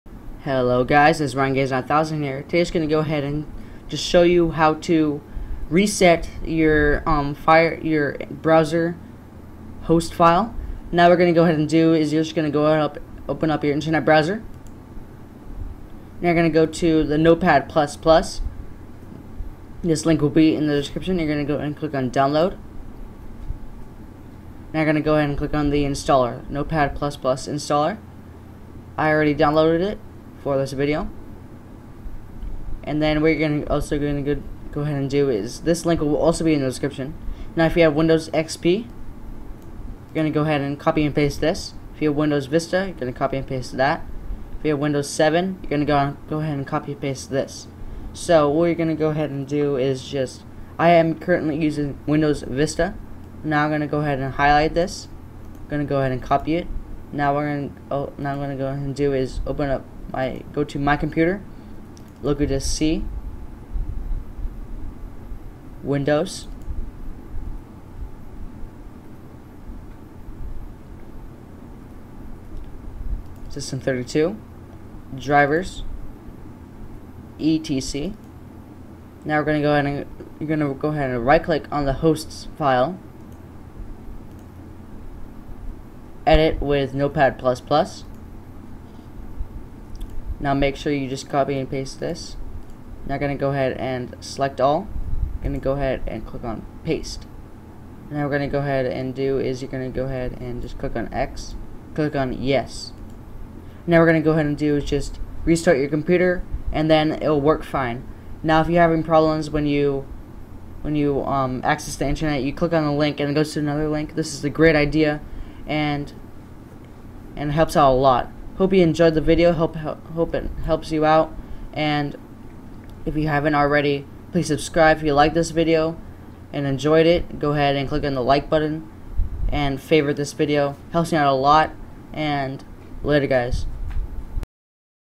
How To Reset Your "Hosts File" (Please Read The Discription)
If You Don't Know About The Hosts File And Experienceing A Problem With Internet Browsing Please Read The Following.

(Applies to: Windows 8, Windows 8.1, Windows 7, Windows Vista, Windows XP & Windows 10)

Resetting Your "Hosts File" Means That When You Are Accsessing The Internet And When You Click On A Link In Google It Brings You To Another Site. This Is Called The "Google Redirect Virus" There Is No Other Way To Get Rid of This. Anti-Virus Software Cannot Detect It, So If You Want To Fix That Problem Fast Follow The Steps I Did In This Video !!!